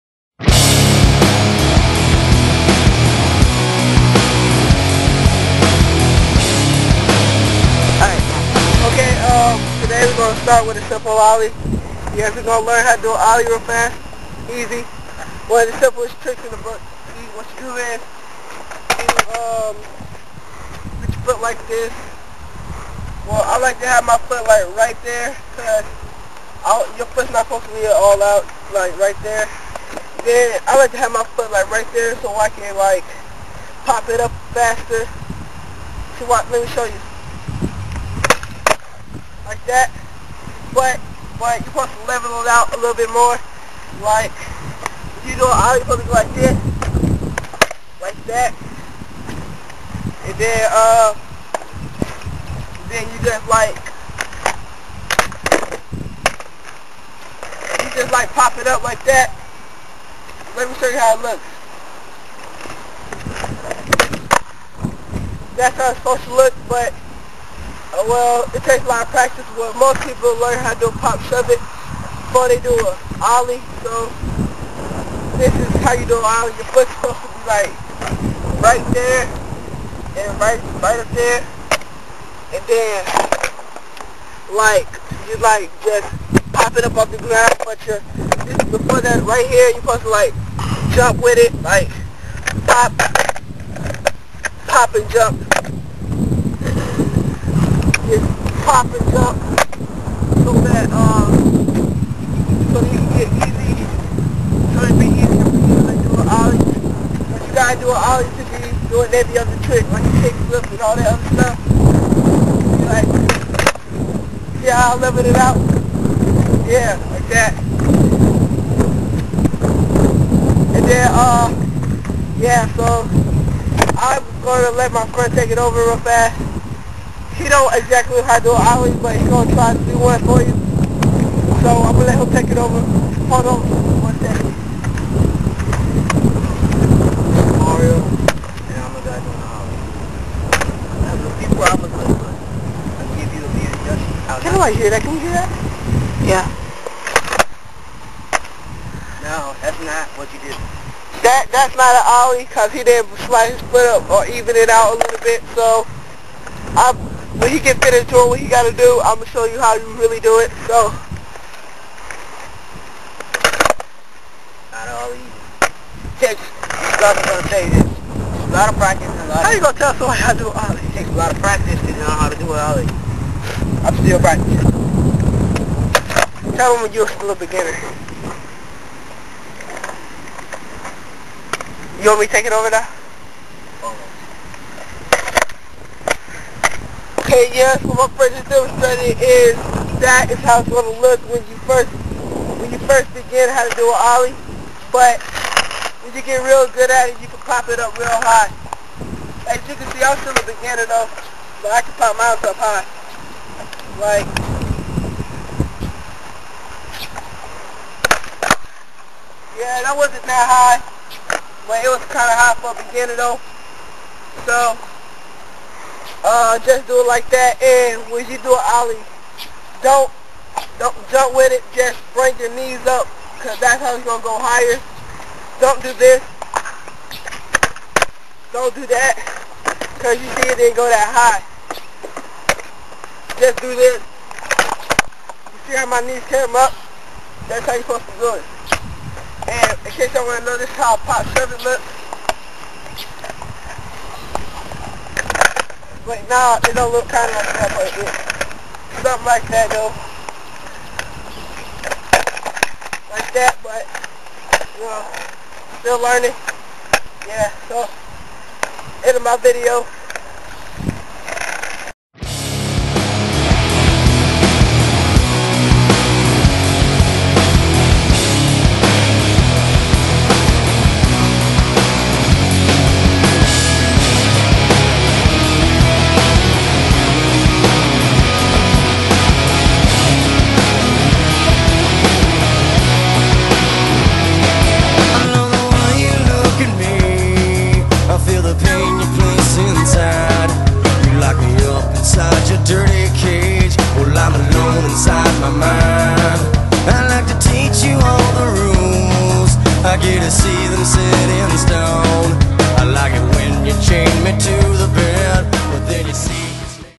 Unstoppable RNM: The Basics of the Ollie
Rashad and Mario teach the do's and don'ts of the ollie on a skateboard